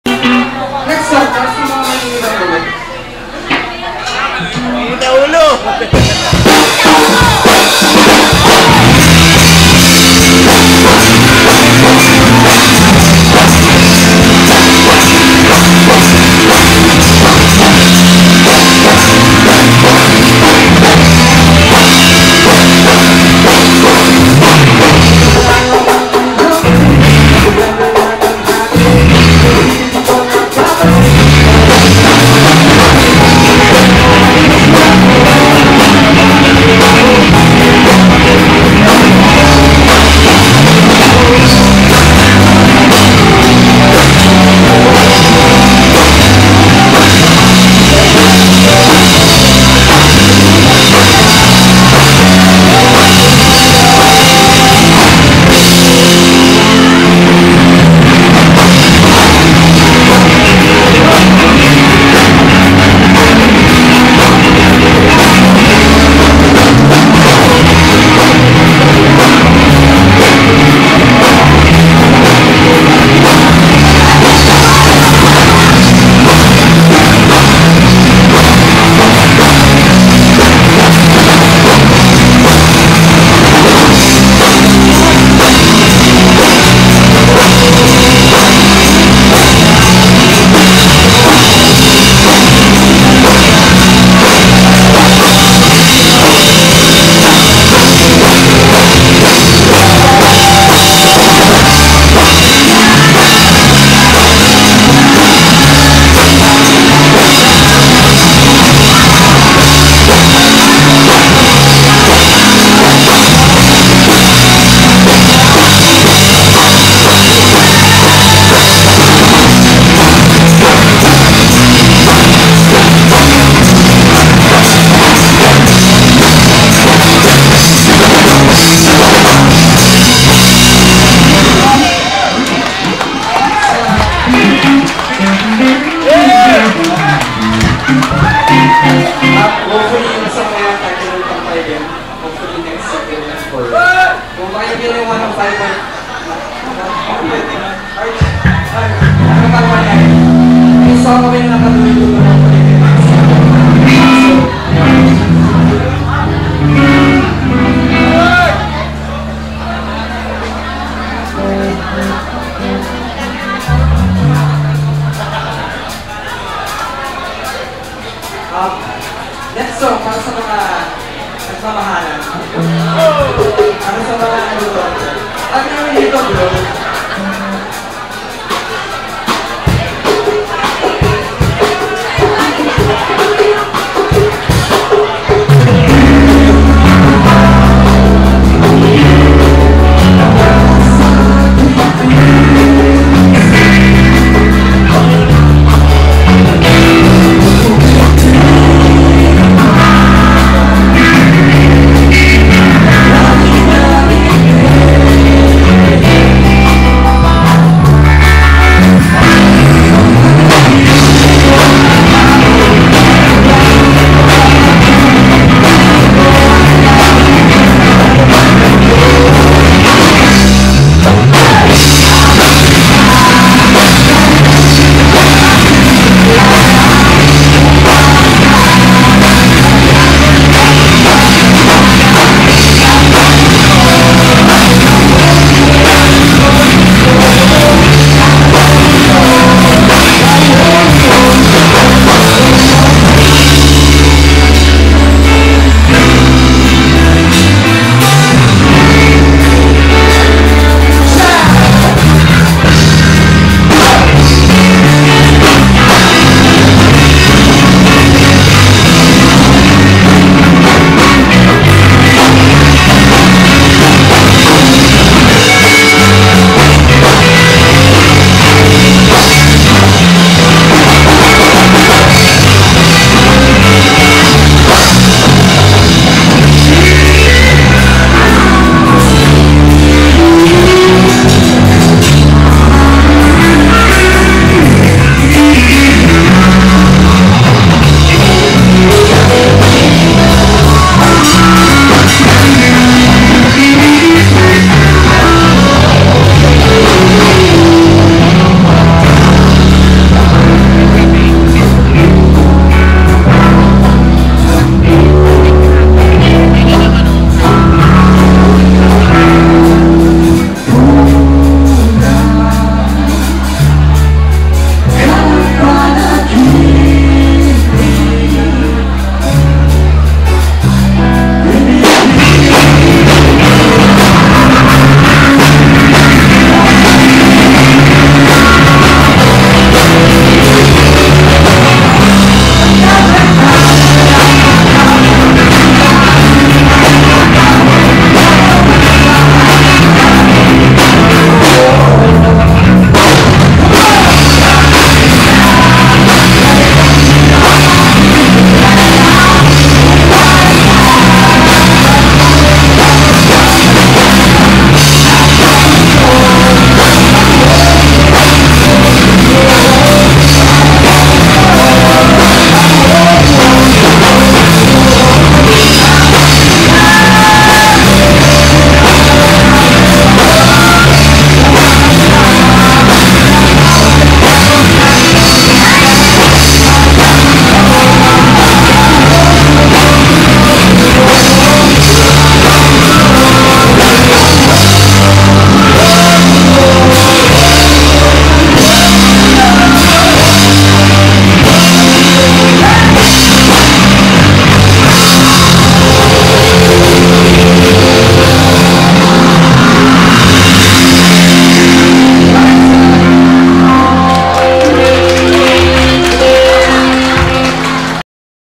KAMPAI Video 2 at Tribute to Francis M Gig.avi
Gurnak Productions in cooperation with Homer's present - The Man from Manila: A Tribute to Francis M Gig, March 5, 2011